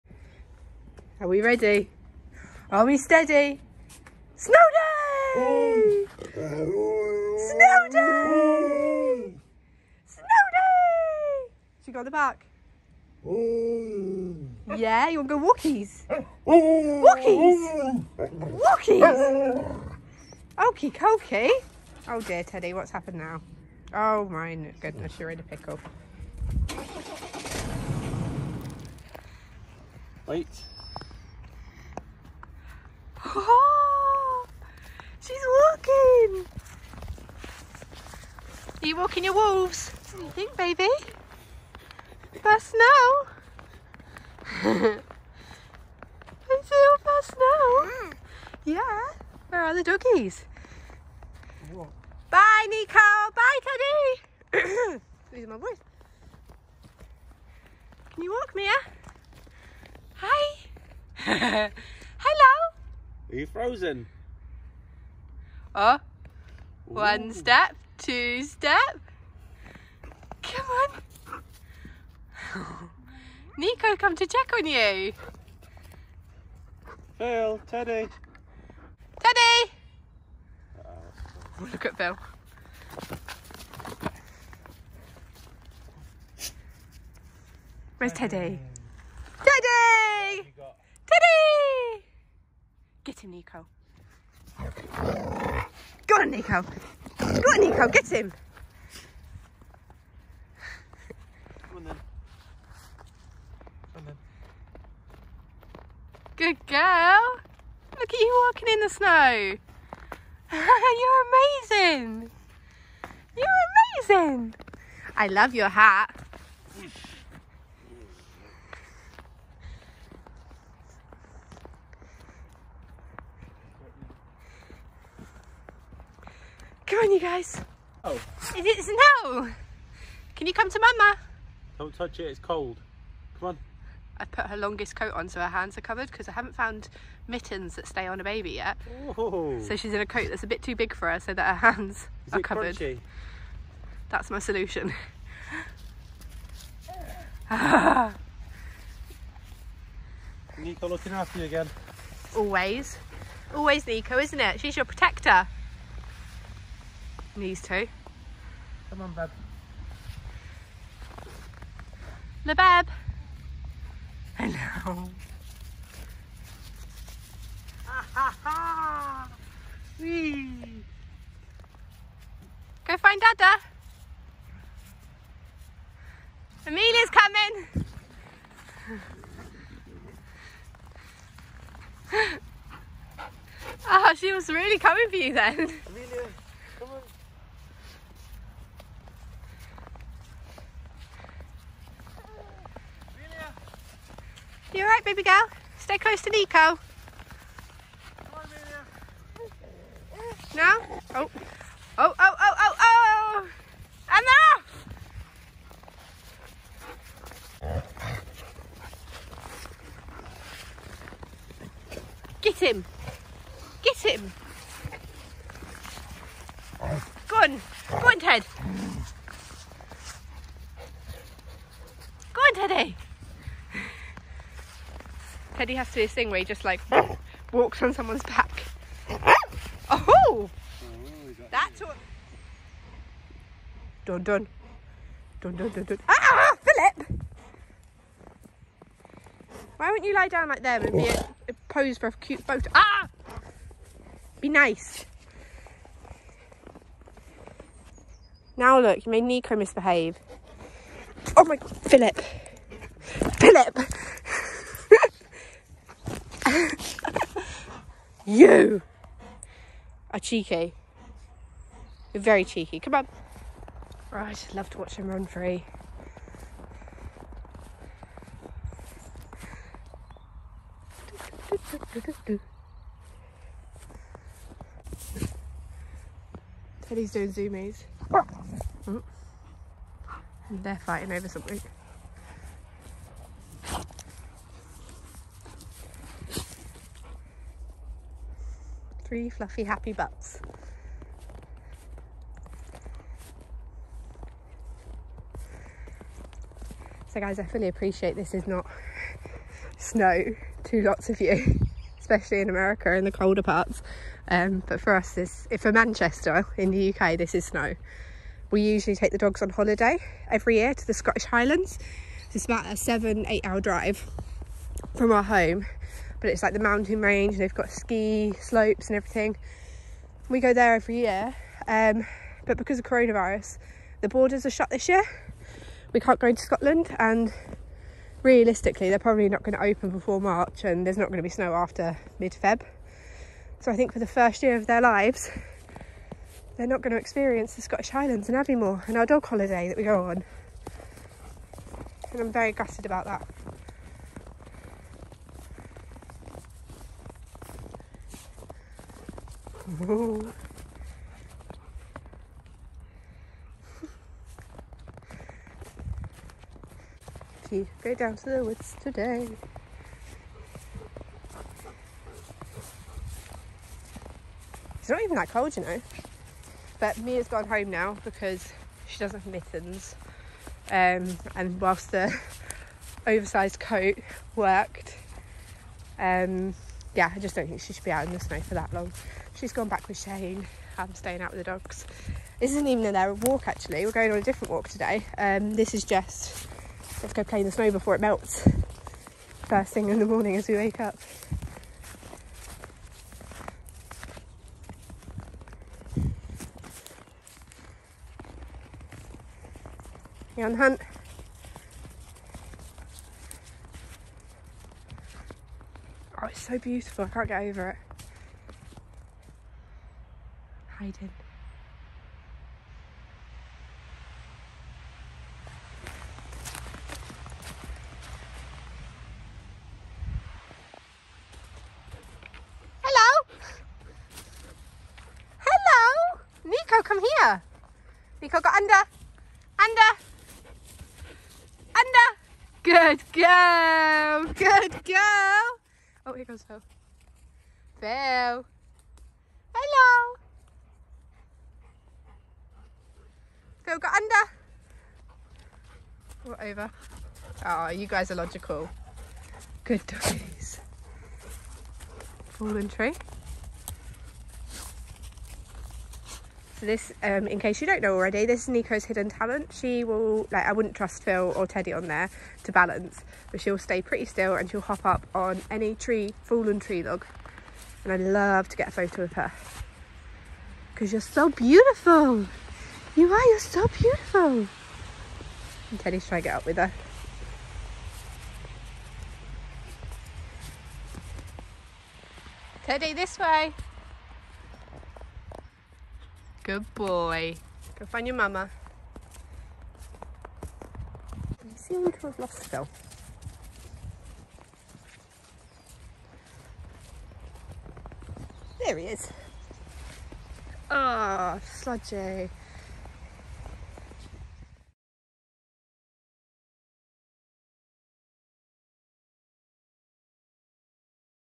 Baby And Wolves Play In The Snow! (Cutest Ever!!)
Hey! So we were lucky enough to get snow for a few days! It's Mia and Teddy's first time in the snow and don't they all just look the cutest. Mia stays close to Niko when she is trying to run after Daddy. Teddy however is getting braver as he gets older and has started wandering off and exploring himself, this is the testing stage where you really have to train recall. Luckily we have a lot of space and can let them all off to roam. These dogs are so grateful for lockdown with Mum and Dad working from home 24/7, milo not so much haha.